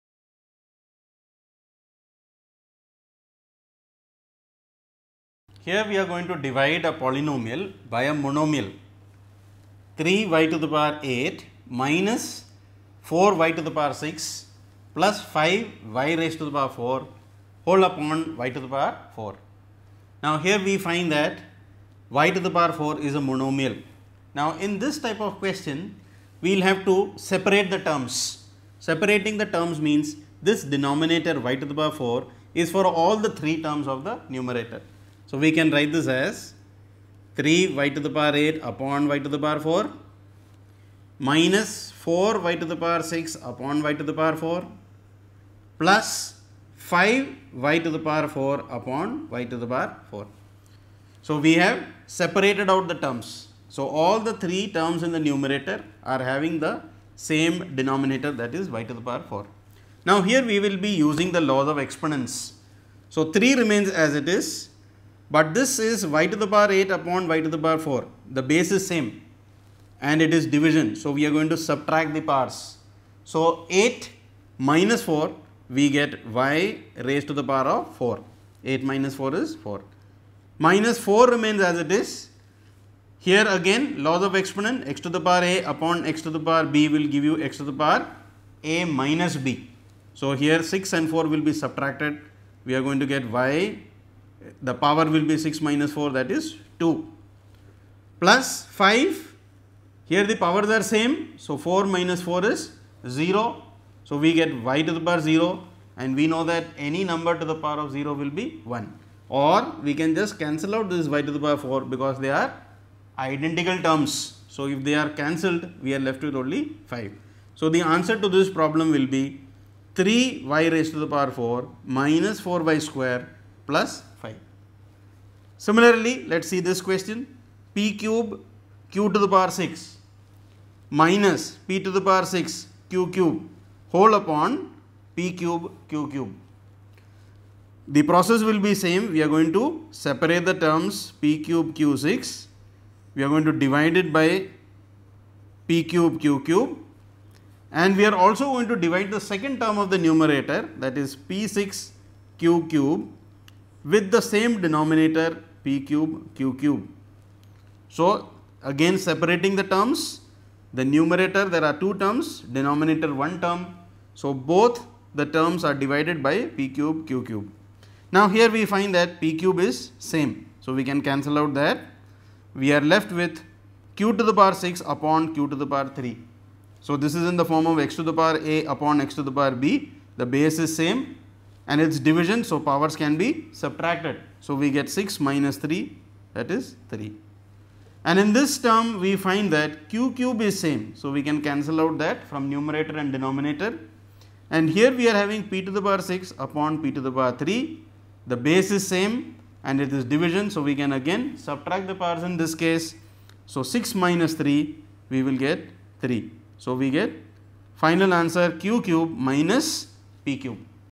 CBSE Class 8 Maths Factorisation Division of Polynomials by Monomial
Chapter: Factorisation
Topic: Division of Polynomials by Monomial